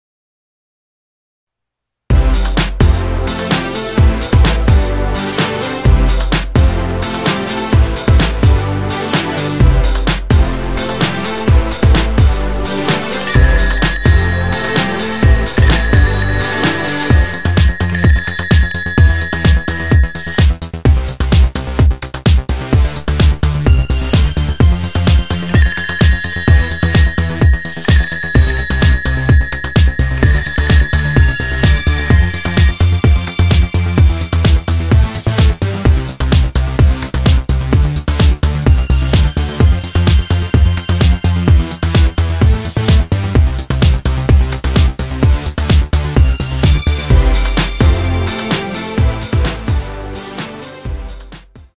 Brinquedo Telefone a Corda com Som e Luz - DM Toys
Todas as crianças adoram brinquedos com luz e sons.

Com esse brinquedo com o formato de telefone a diversão está garantida!!!

Ao apertar as teclinhas do telefoninho ele faz sons e acende luzes!!!

E é só puxar a cordinha na parte de trás do telefone para que ele se movimente para frente!!!